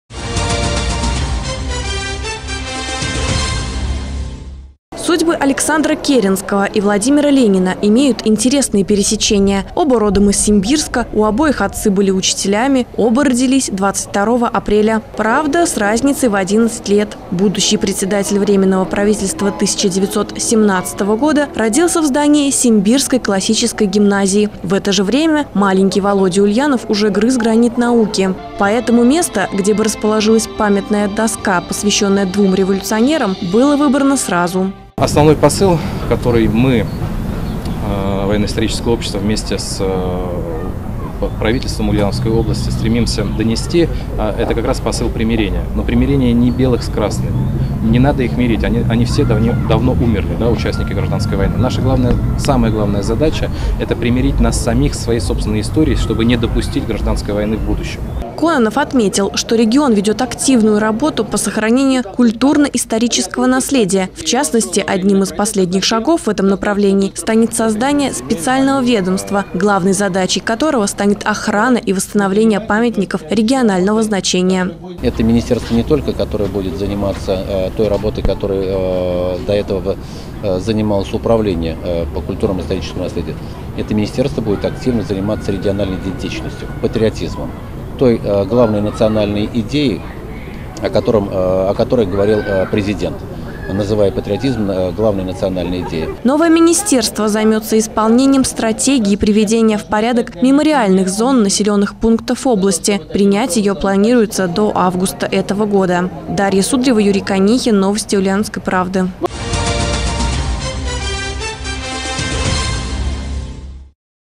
Двум революционерам-землякам посвящается
В Ульяновске открыли памятную доску, посвященную Владимиру Ленину и Александру Керенскому.